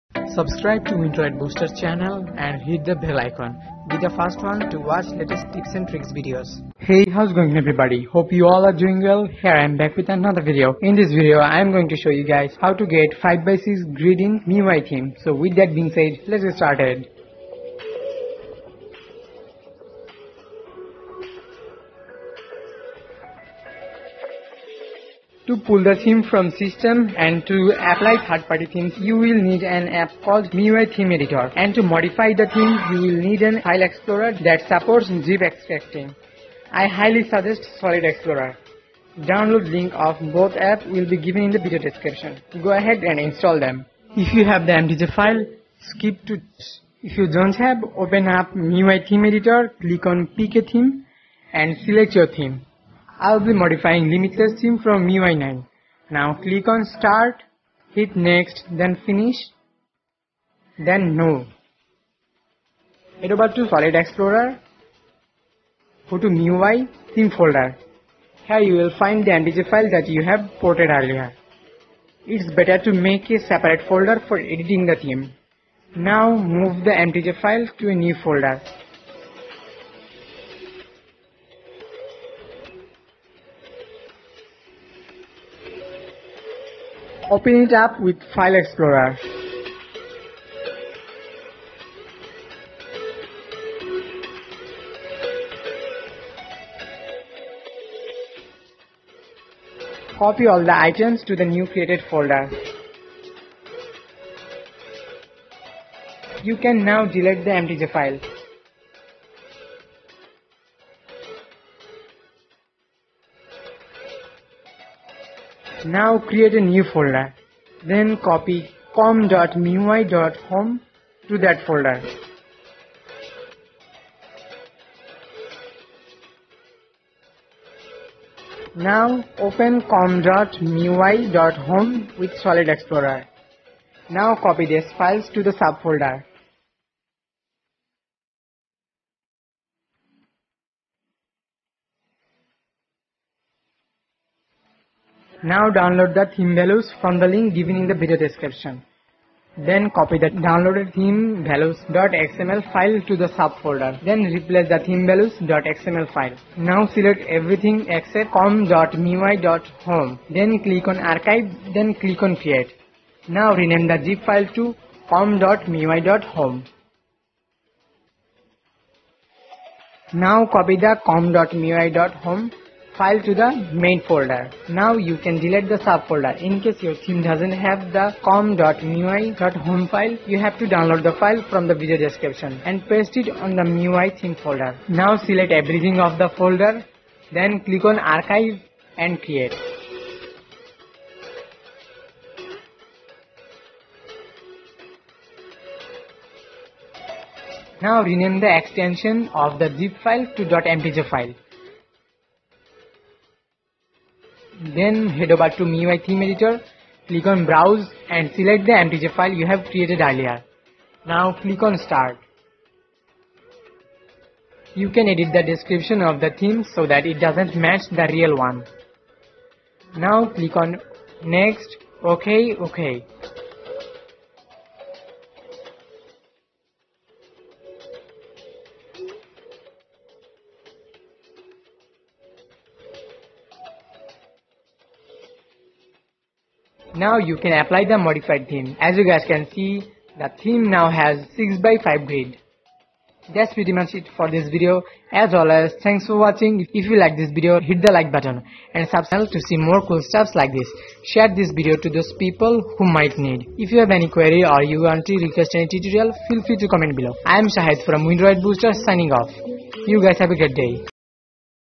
How to get 5*6 app grid in MIUI 8/9 😍
Hey how's going on?
In this video I've shown how to get 5*6 app grid in miui 8/9 homescreen.
To do that you have to modify the theme.